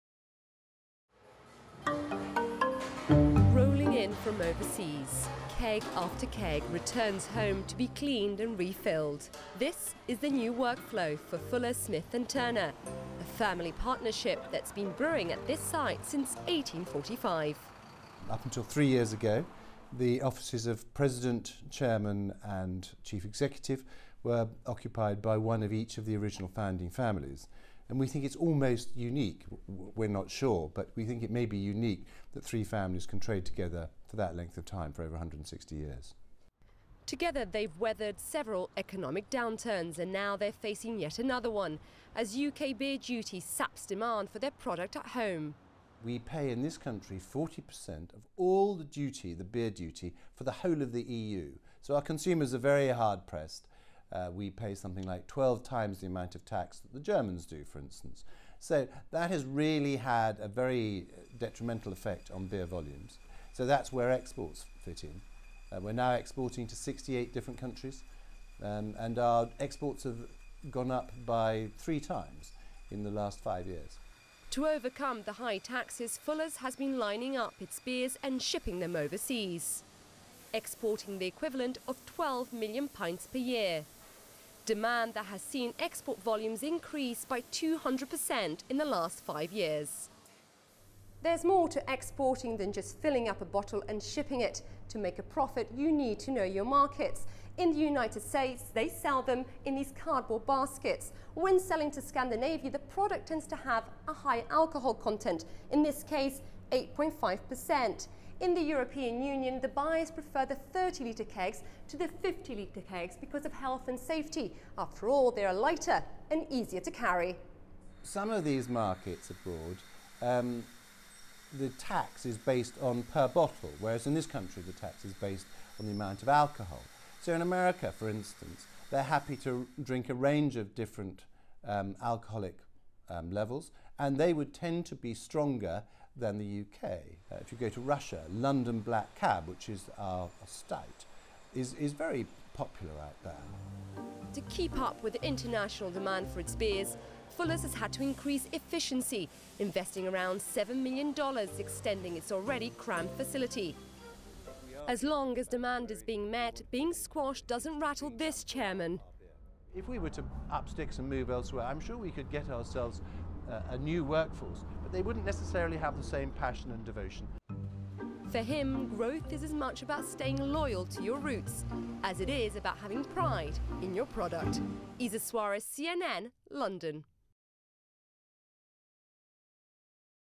UK: HIGH TAXES MAKE BEER-MAKER BREW ABROAD
One in five pints produced by London Pride-owner Fuller Smith & Turner at its historic brewery in London is now shipped abroad as Britain's traditional beer companies are forced to seek out new sources of income to overcome high taxes back home. CNN's Is.